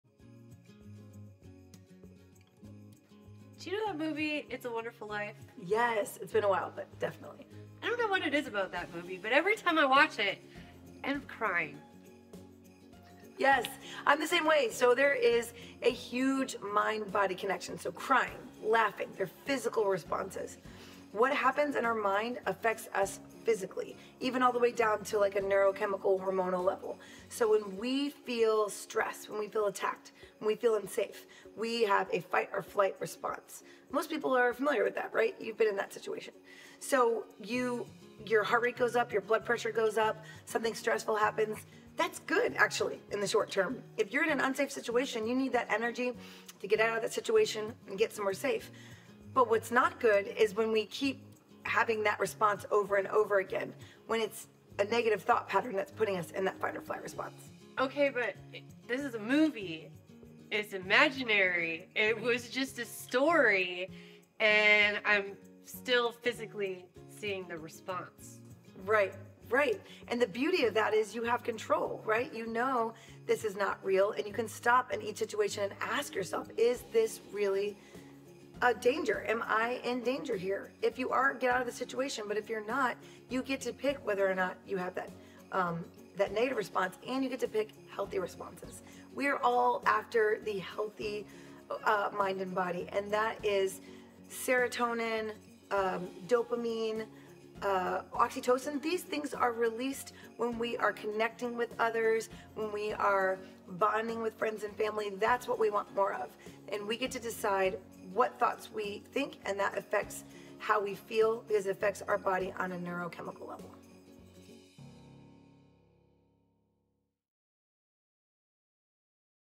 Story 3 I'll Have A Blue Xmas
The holidays are about peace and love. Yet, many of us experience stress and negative emotions. In this seven day series, Erica Humbert APRN and Heidi Aderman Performance Coach explore the holiday stories we tell ourselves that are causing stress.

In this video; we talk about the mind and body connection. Why do we cry when we watch a holiday movie? What causes that feeling of giddy excitement when we get together with friends at our holiday parties? How to get more of the happy and the healthy feelings this Holiday season?

We help people reduce stress everyday. Learn more about us here.